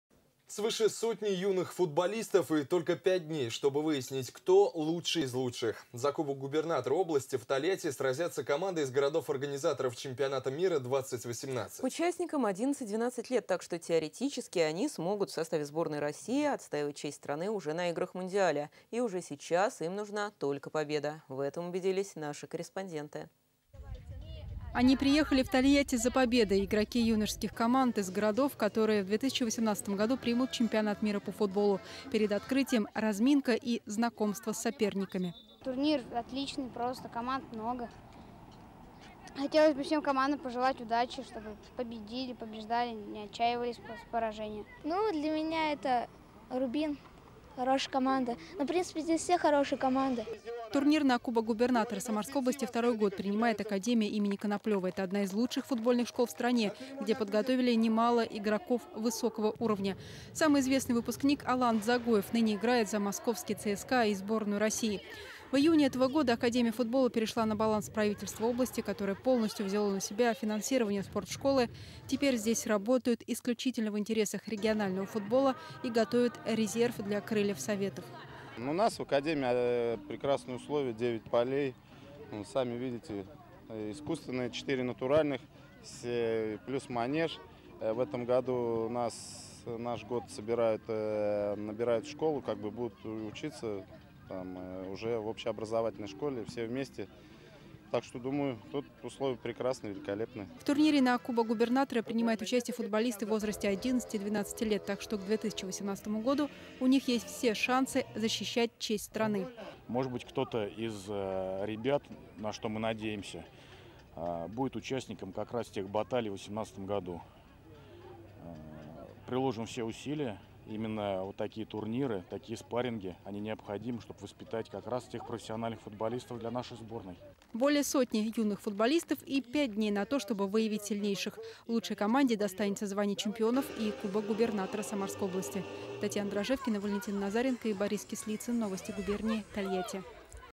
Кубок губернатора по футболу в Тольятти
Свыше сотни юных футболистов и только пять дней, чтобы выяснить кто лучший из лучших. За кубок губернатора области в Тольятти сразятся команды из городов-организаторов чемпионата мира 2018. Участникам - 11-12 лет, так что теоретически они смогут в составе сборной России отстаивать честь страны уже на играх мундиаля. И уже сейчас им нужна только победа - в этом убедились наши корреспонденты.